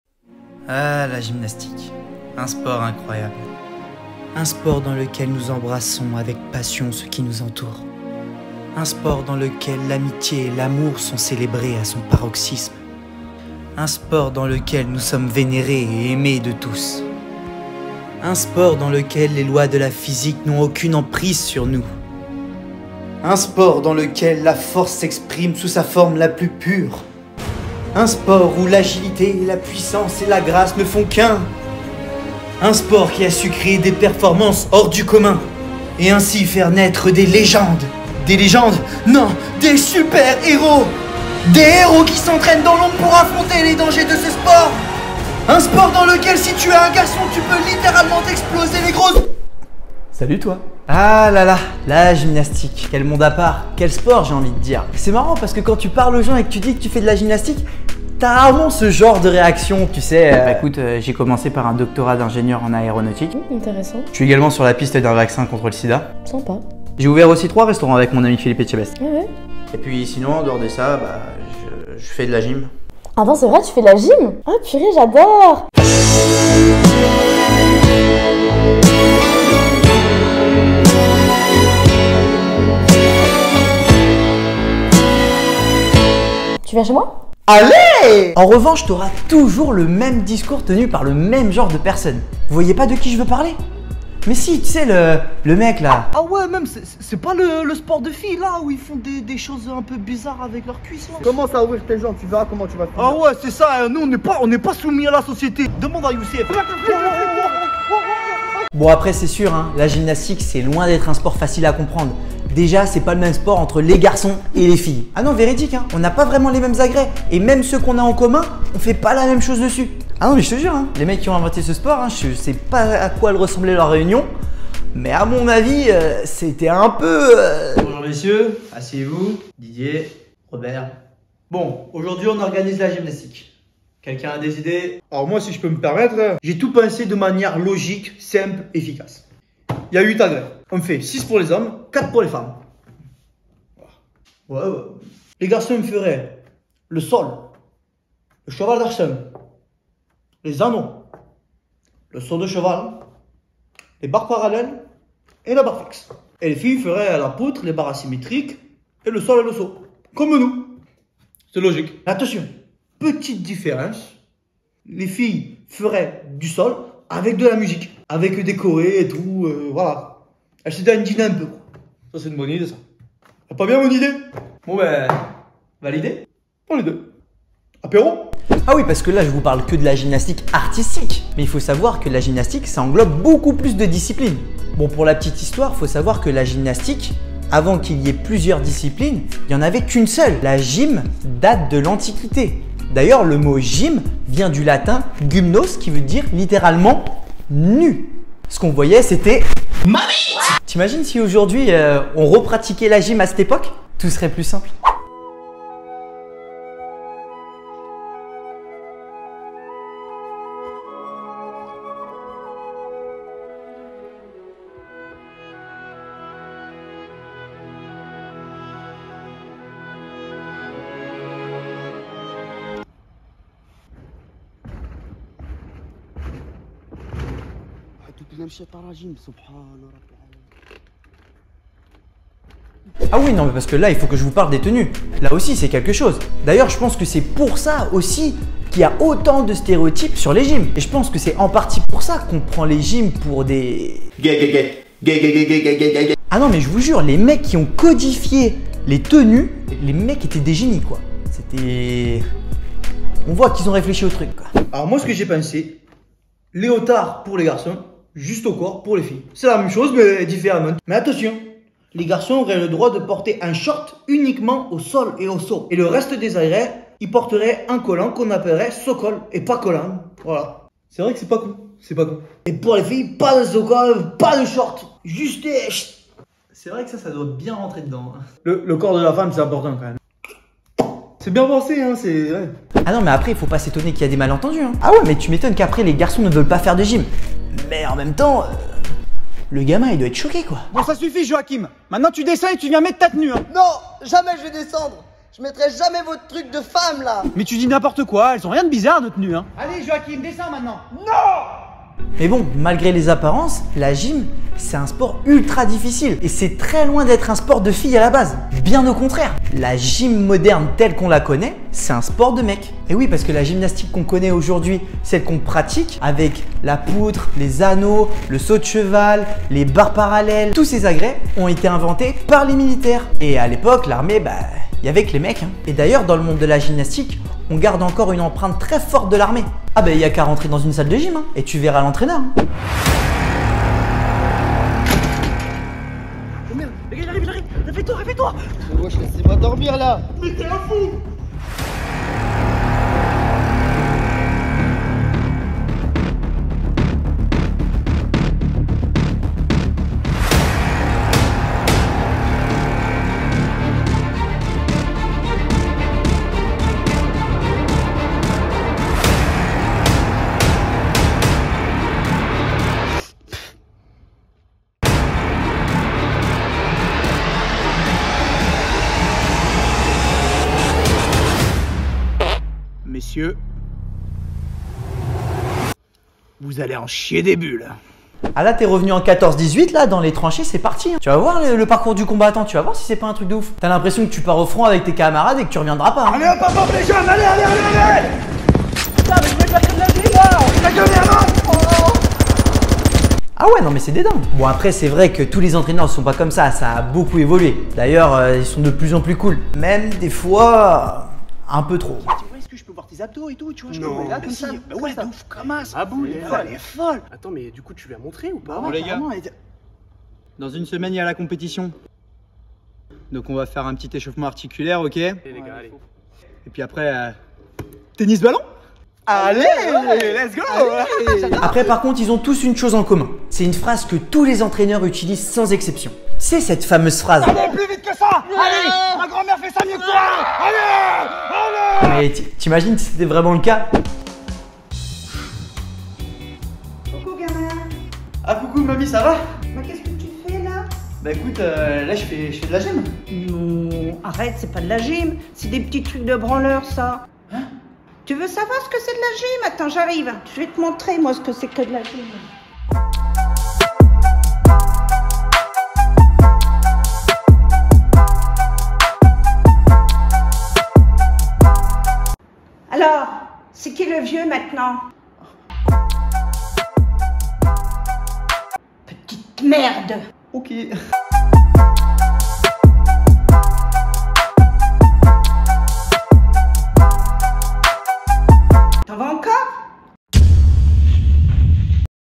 LA GYMNASTIQUE
Tous les sports ont le droit à leur sketch, même la gymnastique.

Réalisateur: Sebastien Lambert
Écrit par: Sébastien Lambert
Production: Sébastien Lambert
Monteur: Sébastien Lambert

Avec la participation de: Alisson Lapp, Noé Langlois, Hamza Hossaini, Lorenzo Pambianchi et Super Mamie